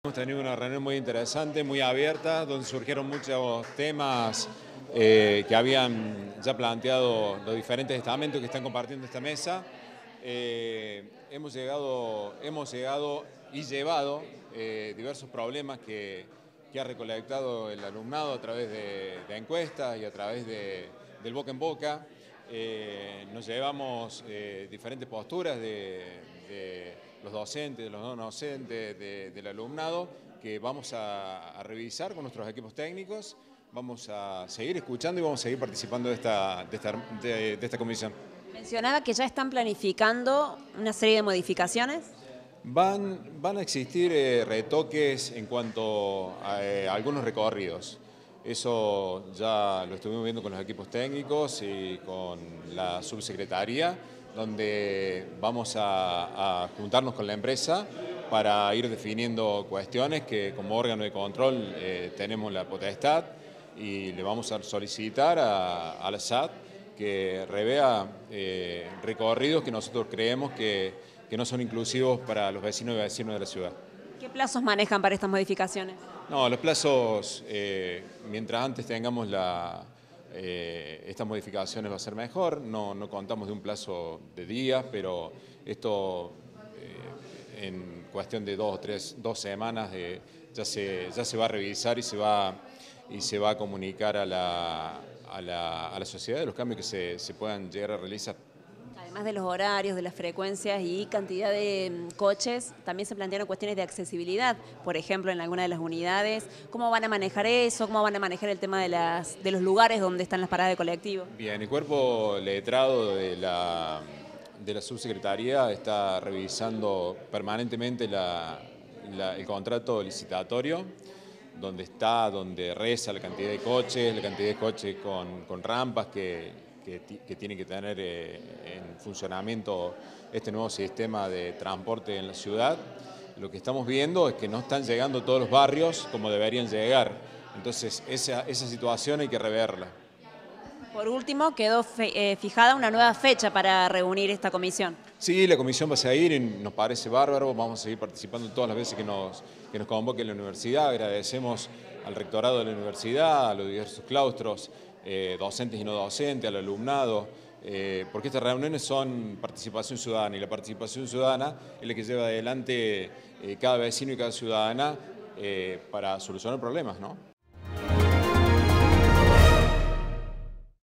NUEVO SISTEMA DE TRANSPORTE: INTEGRANTES DE LA COMUNIDAD UNIVERSITARIA MANIFESTARON DISCONFORMIDAD
DIEGO MORENO – SUBSECRETARIO MUNICIPAL DE TRANSPORTE